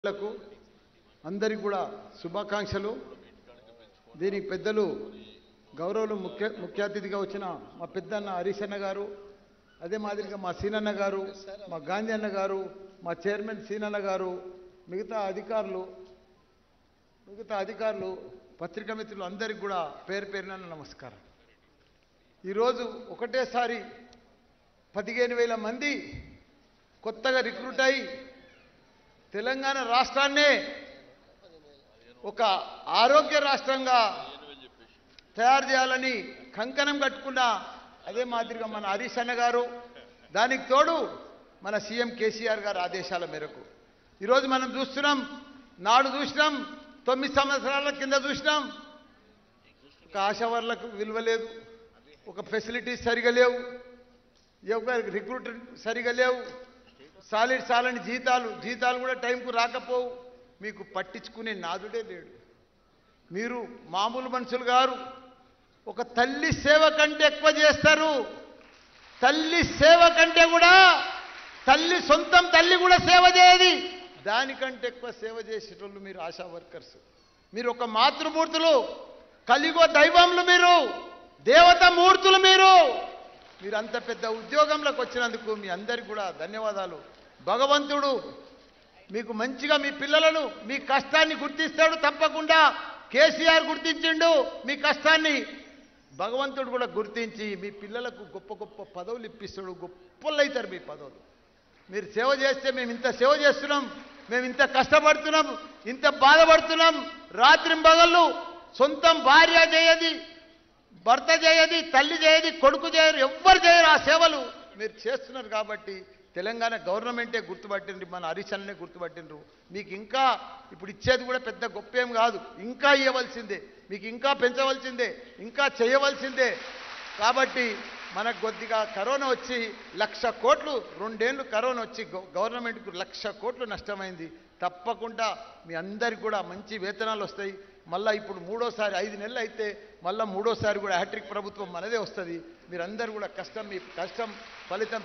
తల్లి సేవకంటే ఎక్కువ చేస్తారు..! | Minister Malla Redyy About Asha Workers
తల్లి సేవకంటే ఎక్కువ చేస్తారు..! | Minister Malla Redyy About Asha Workers | Hit TV News